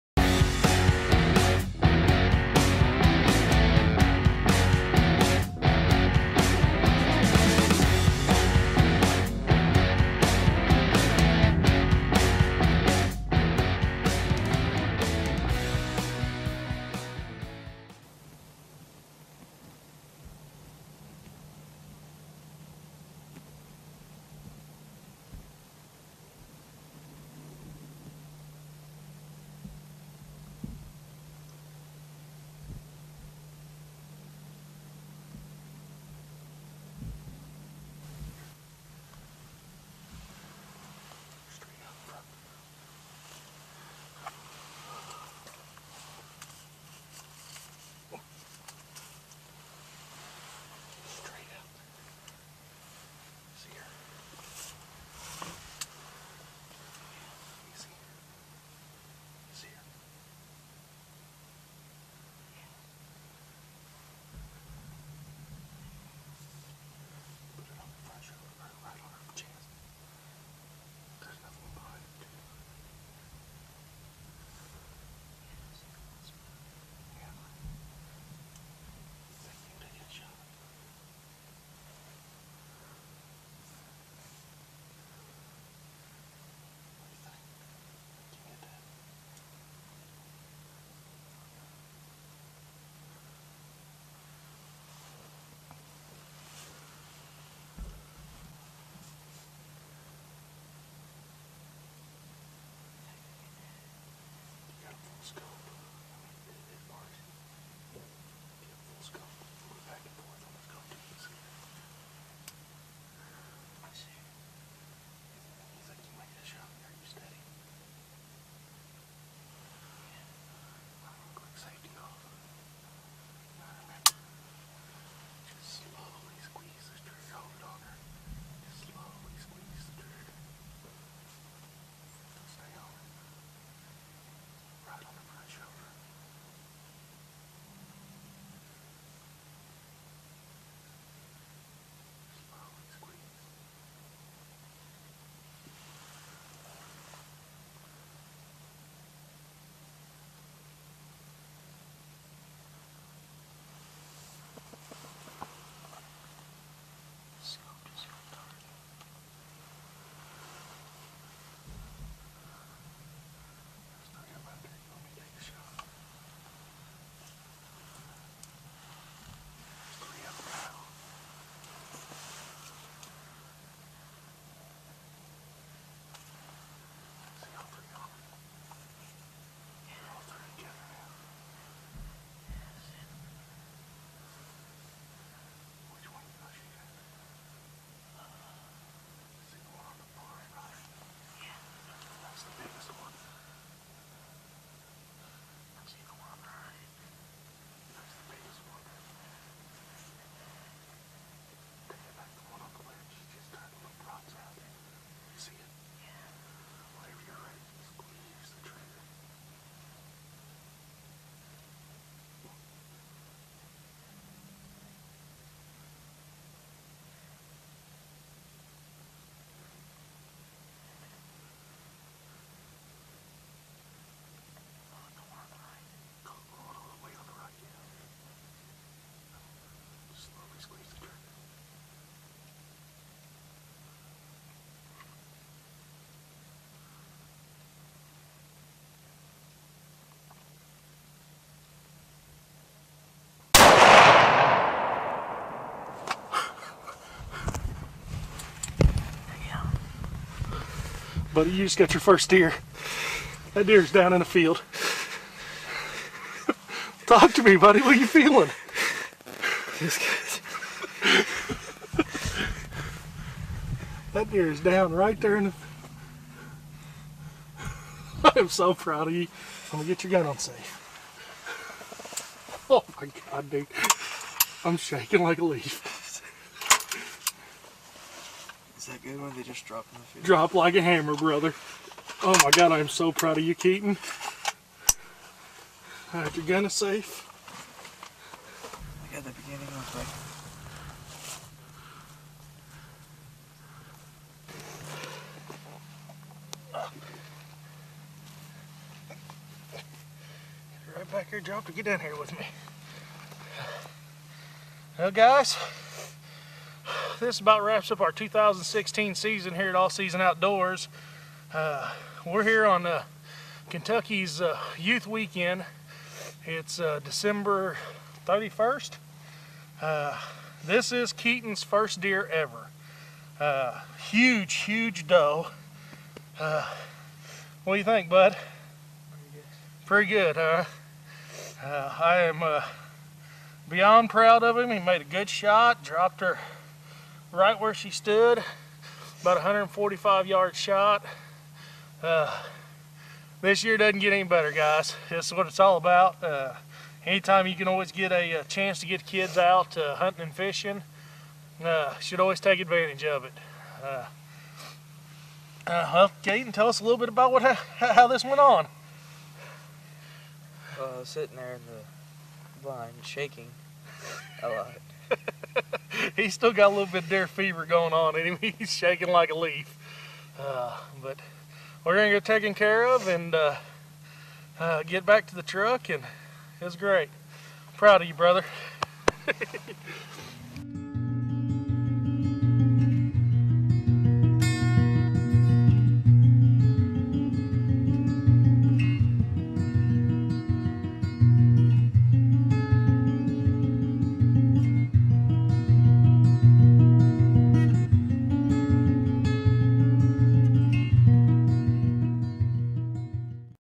Keaton Nagel's 2016 Deer
Keaton Nagel's first deer. Kentucky's free youth weekend 2016